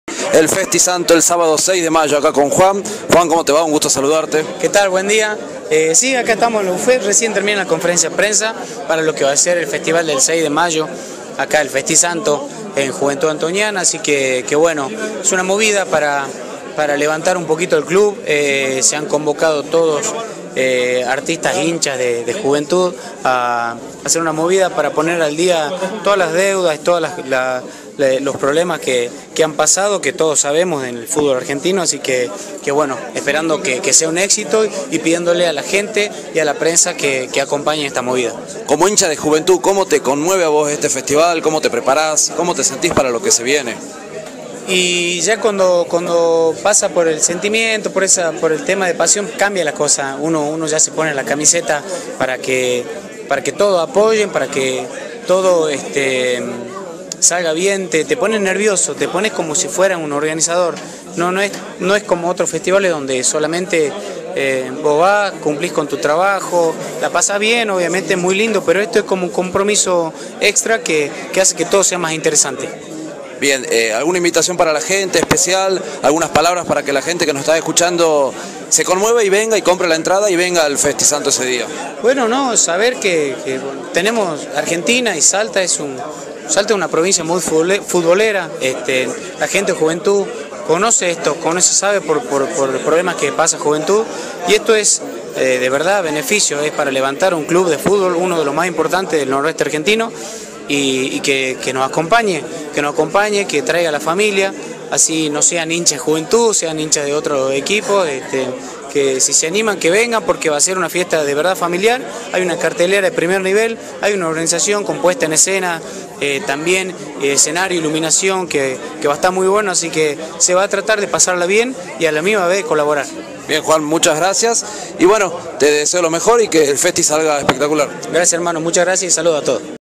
JUAN PEÑALVA, CANTO CUATRO LANZAMIENTO DEL FESTISANTO EL PRÓXIMO 6 DE MAYO
JUAN PEÑALVA, CANTO CUATRO
LANZAMIENTO DEL FESTISANTO EL PRÓXIMO 6 DE MAYO EN EL ESTADIO FRAY HONORATO PISTOIA